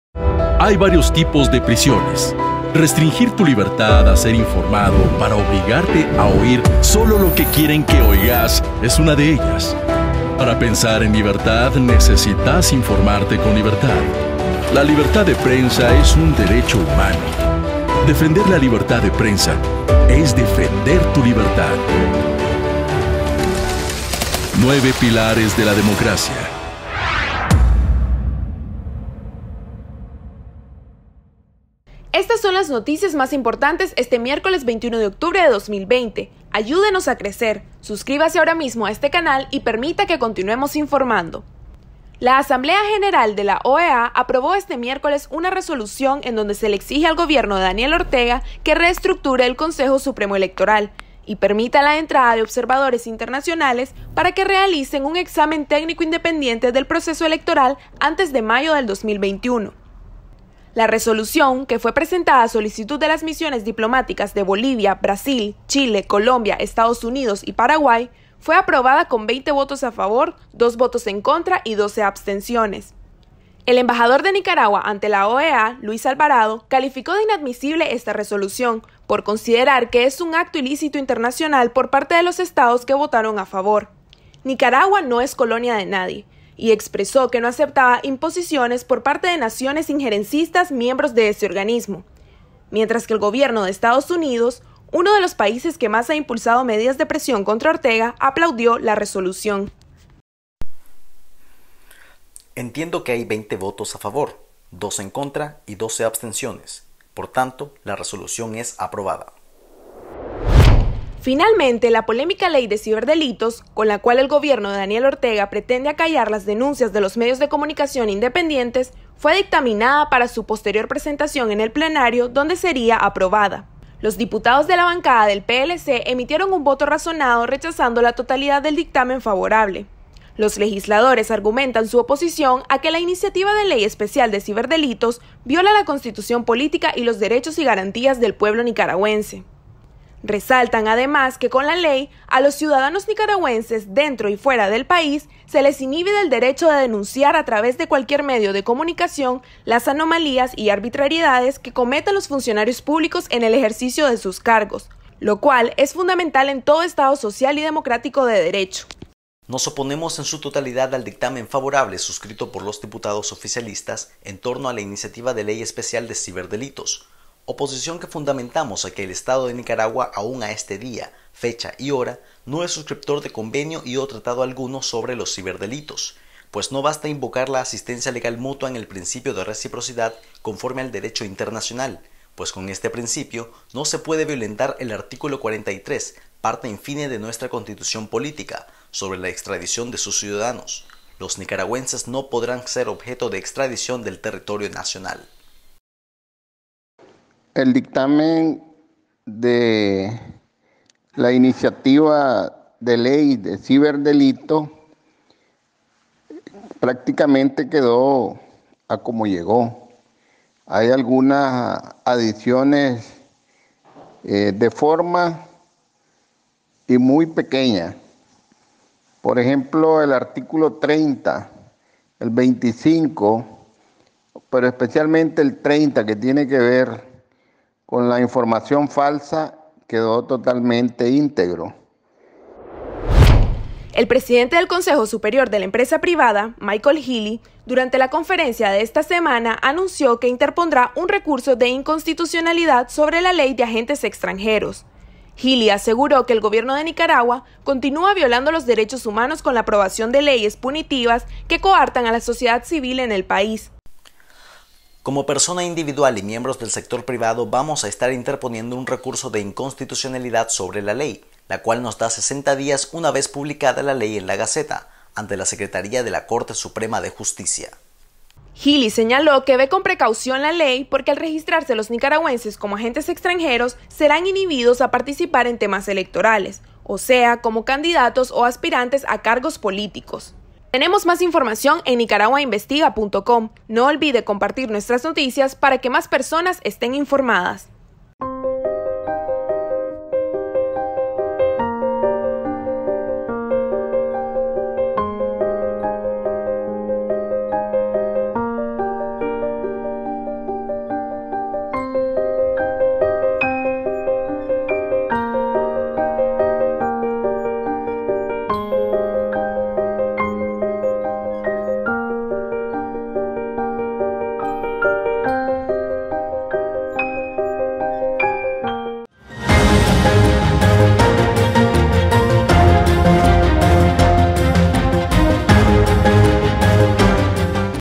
LoÚltimo |🔺⚠ Noticias de Nicaragua miércoles 21 de octubre de 2020
Estas son las noticias más importantes de hoy:

▶ Aprobación contundente de OEA sobre Nicaragua
▶ Dictaminan Ley de Ciberdelitos
▶ Cosep: Ley de Agentes Extranjeros bloquea a posibles candidatos presidenciales